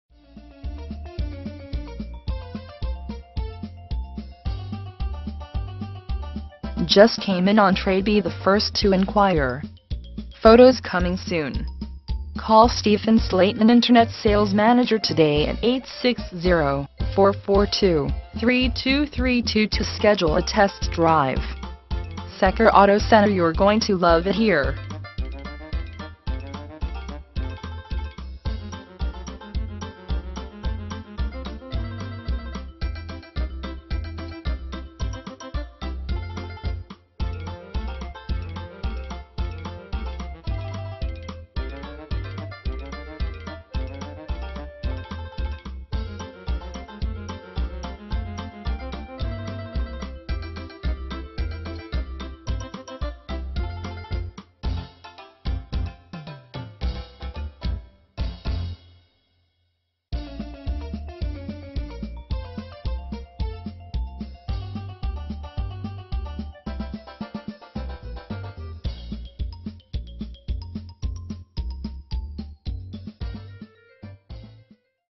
2007 Audi A3 - Secor Volvo Mitsubishi Lotus SAAB - New London, CT 06320
Secor Volvo Mitsubishi Lotus SAAB
575 Broad Street

For Sale: 2007 Audi A3
VIN: WAUKD78P37A027282
Engine: V6 Cylinder Engine 3.2L/195
Drivetrain: All Wheel Drive
Transmission: Automatic
Mileage: 117,496
Color: Lava Gray Pearl (exterior) Stone/Lt Gray (interior)

Features:
* Just came in on trade* Be the first to inquire. Secor Auto Center 'You're going to love it here!'